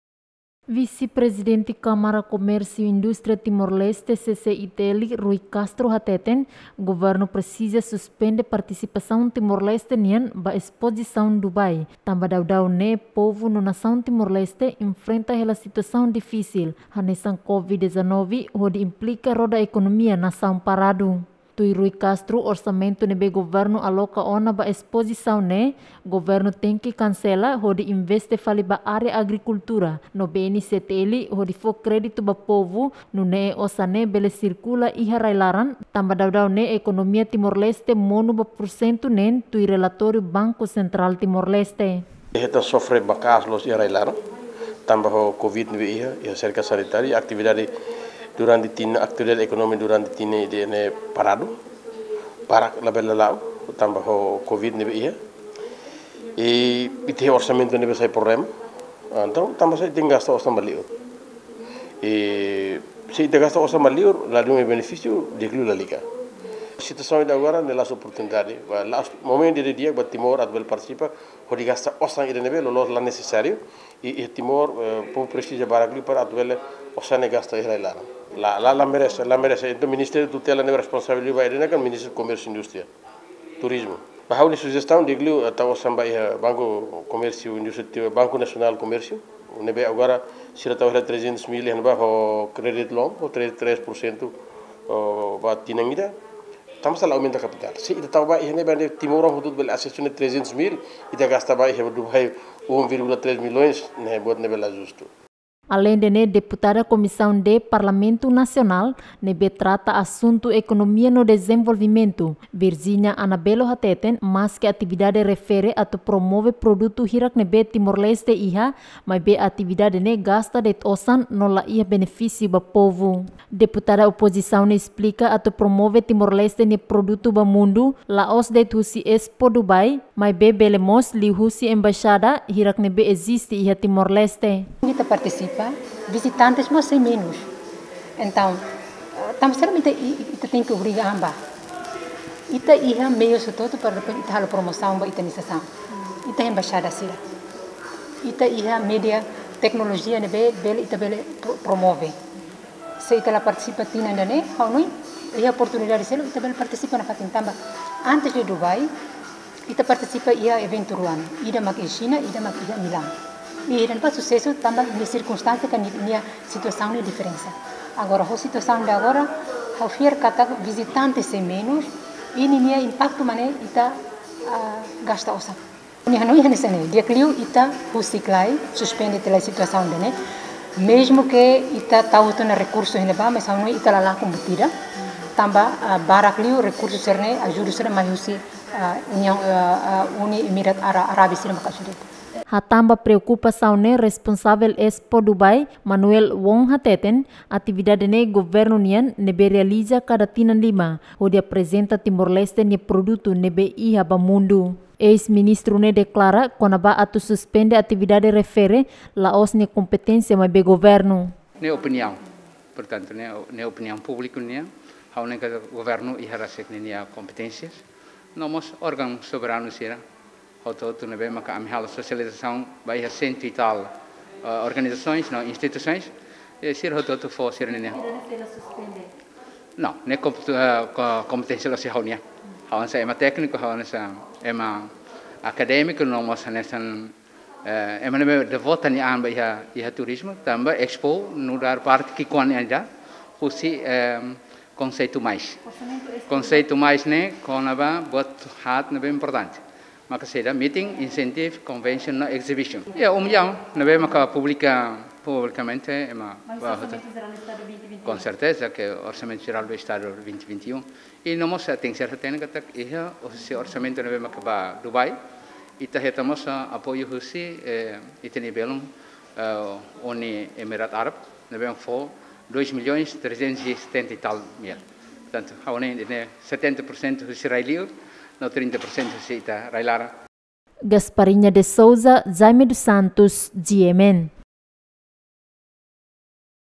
Rui Castro: Timor Leste la merese atu partisipa iha Expo Dubai
Rui Castro hateten Timor Leste la merese atu partisipa iha Expo Dubai tanba povu infrenta hela situasaun difísil